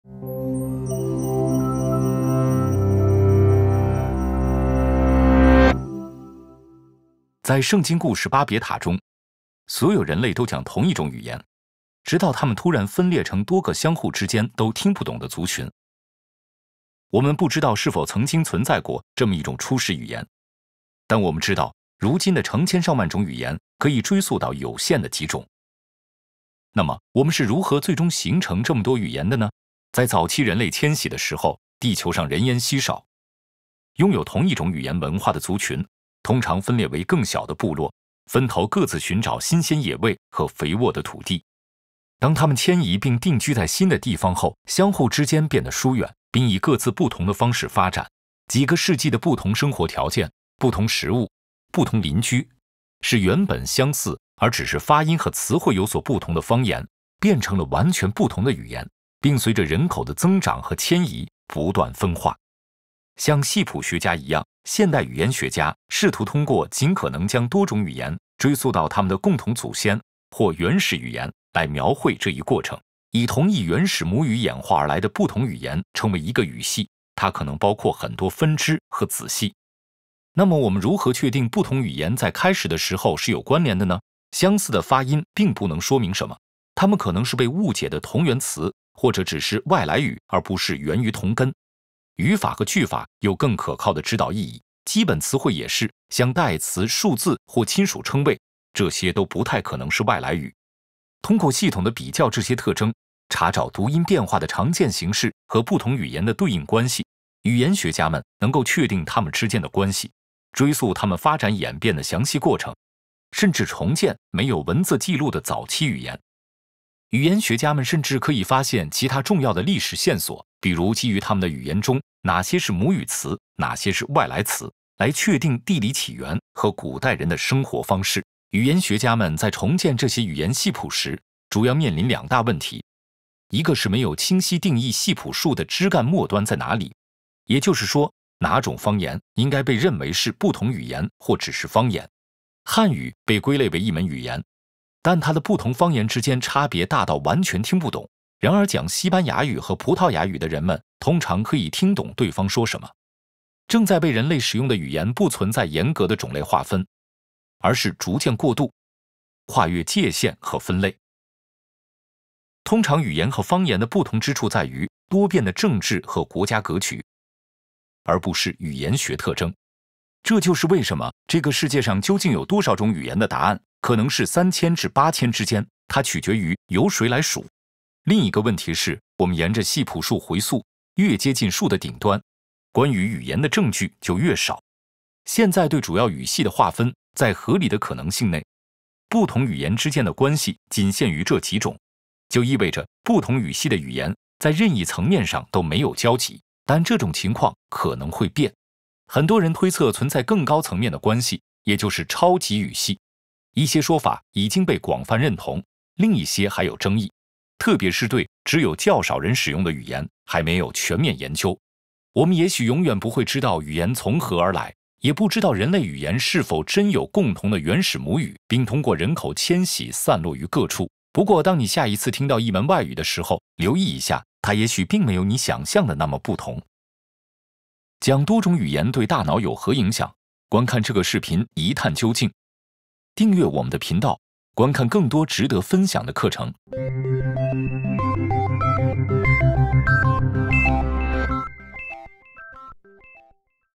语言的演化 - Alex Gendler
在人类历史长河中，很少数的几种语言演变出了数千种语言。这么多的语言是如何演变出来的？ 我们又如何记录所有语言呢？ Alex Gendler 将介绍语言学家如何将语言分成不同语系，并演示这些语系如何为我们提供关于历史的重要洞见。

课程老师 Alex Gendler，导演 Igor Coric。